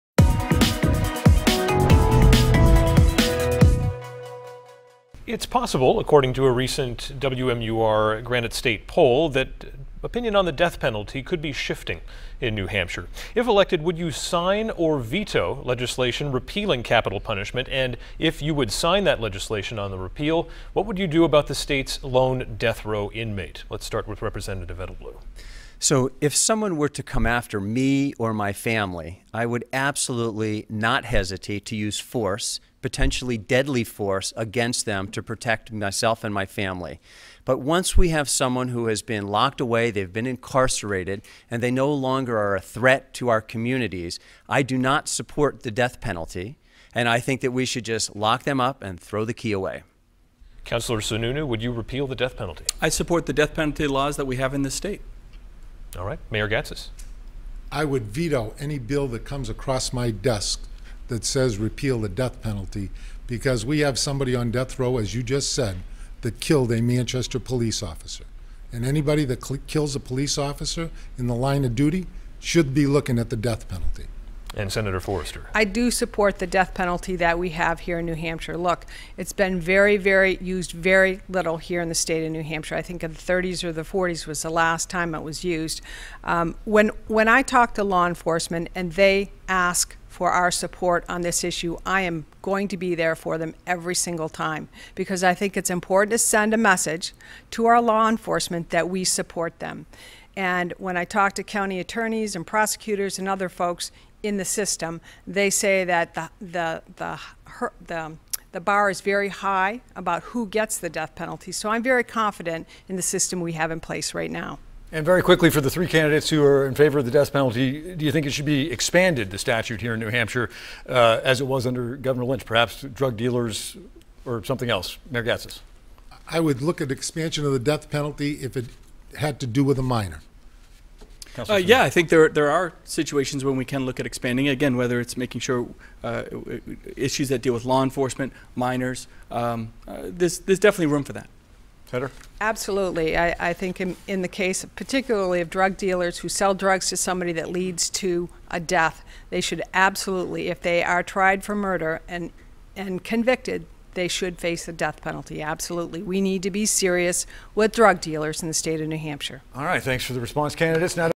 2016 GOP gubernatorial debate: Death penalty
The Republican candidates for governor discuss the death penalty in their Granite State Debate before next week's primary.